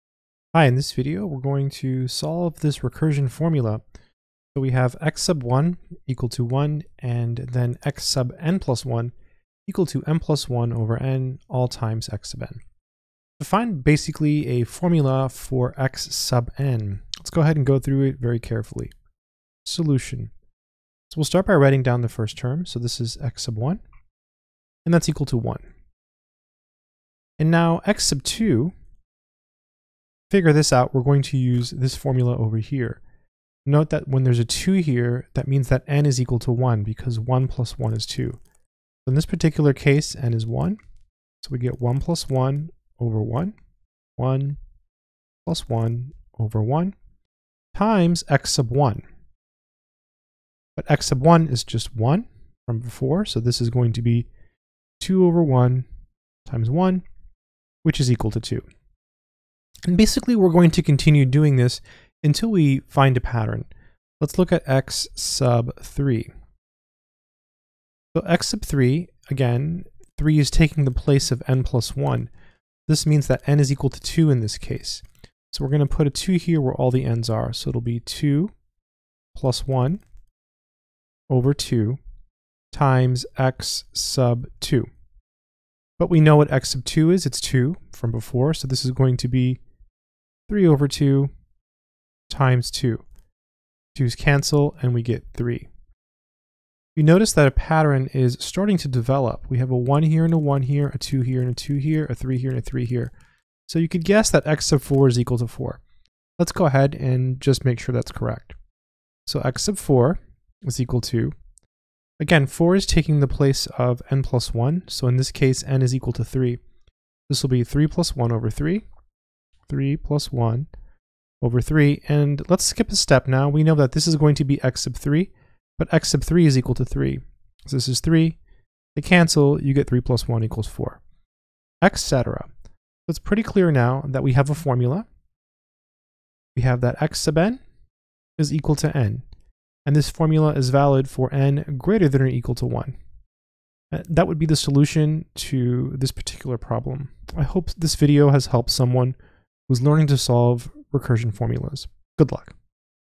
Find a formula for x_n given the recursive definition: x_1 = 1, x_(n + 1) = (n + 1)x_n/n
Find a Formula for x_n given: x_1 = 1, x_(n + 1) = (n + 1)x_n/n